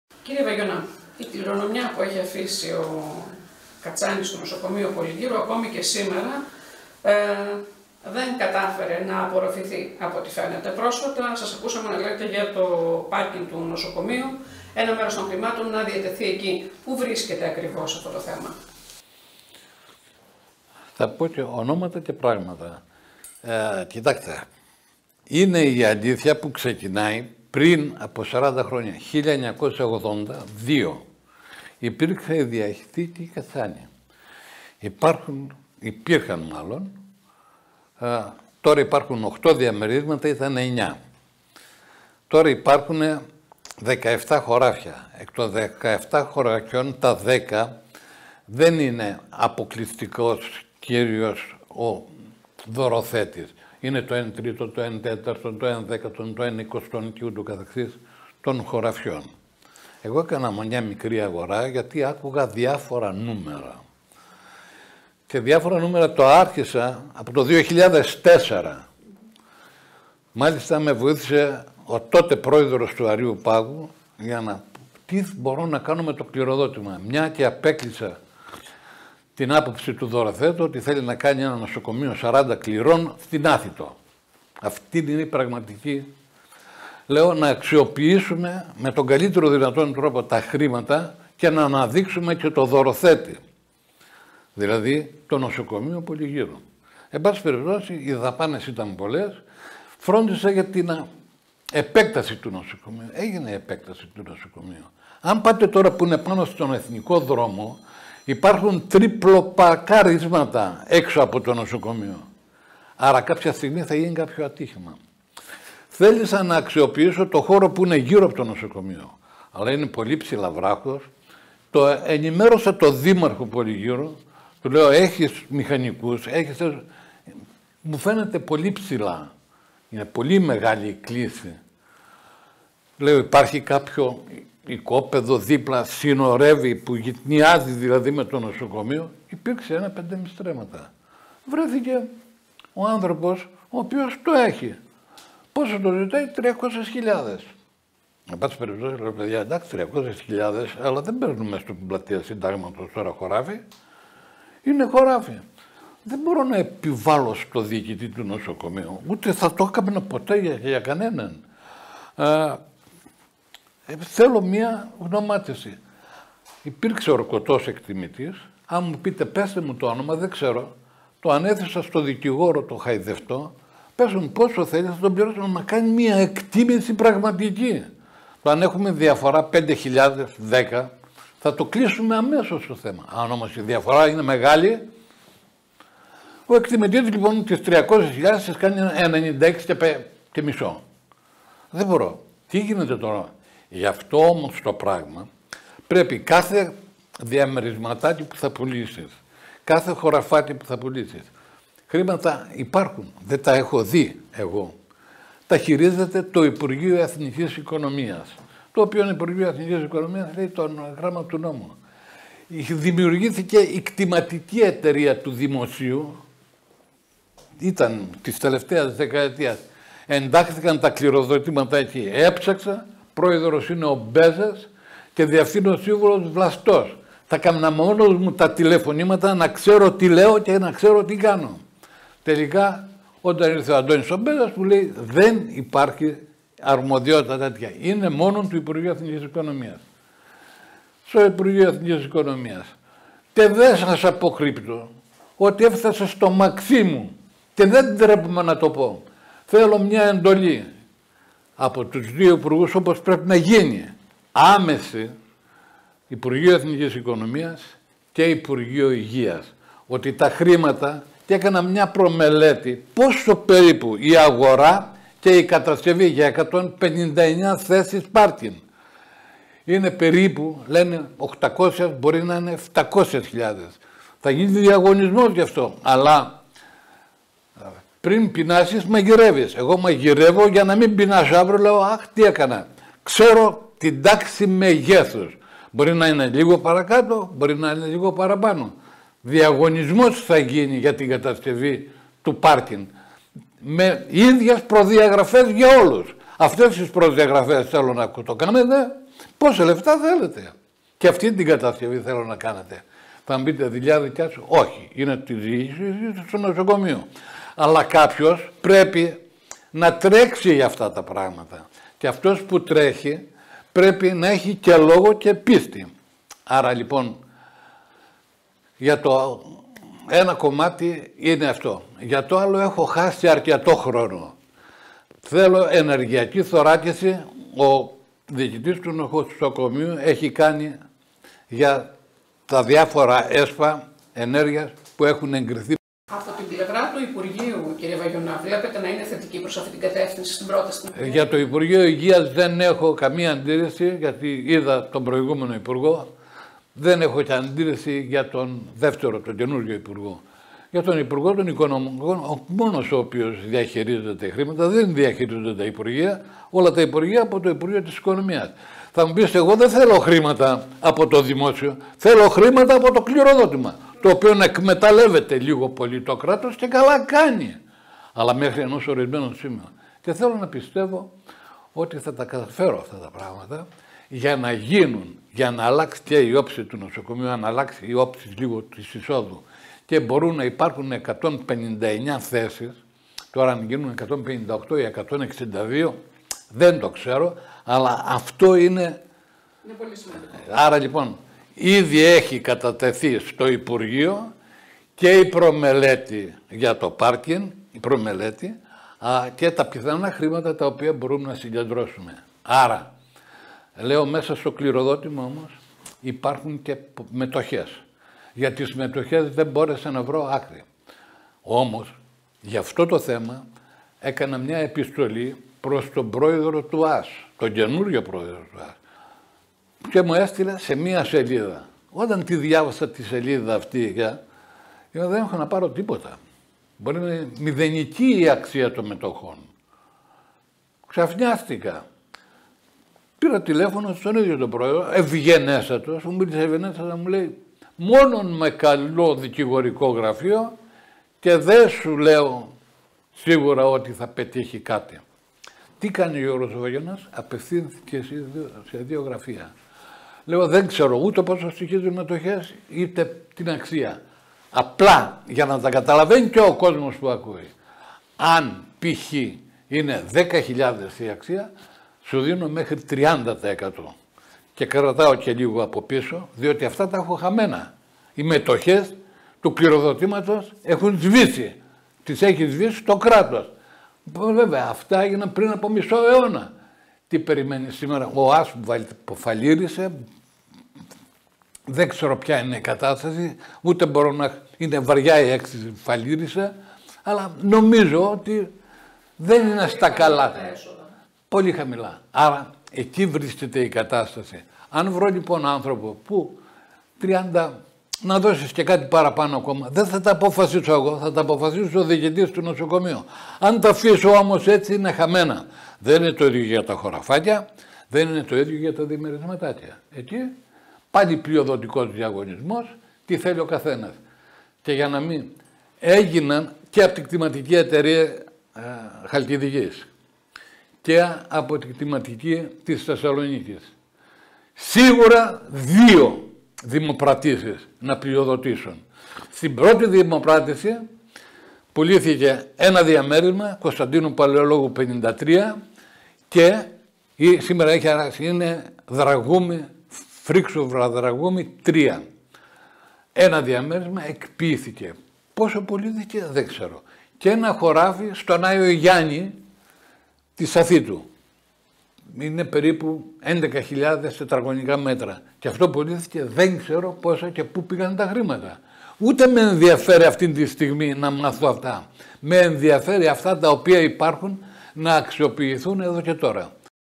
ΤΙ ΑΠΕΓΙΝΕ ΤΟ ΚΛΗΡΟΔΟΤΗΜΑ ΤΟΥ ΚΑΤΣΑΝΗ ΑΠΑΝΤΑ Ο ΒΟΥΛΕΥΤΗΣ ΓΙΩΡΓΟΣ ΒΑΓΙΩΝΑΣ 30,9,21
ΤΙ ΑΠΕΓΙΝΕ ΤΟ ΚΛΗΡΟΔΟΤΗΜΑ ΤΟΥ ΚΑΤΣΑΝΗ ΑΠΑΝΤΑ Ο ΒΟΥΛΕΥΤΗΣ ΓΙΩΡΓΟΣ ΒΑΓΙΩΝΑΣ ΣΤΗ ΣΟΥΖΑΝΑ ΚΑΖΑΚΑ ΚΑΙ ΣΤΟ  XALKIDIKI POLITIKI 30,9,21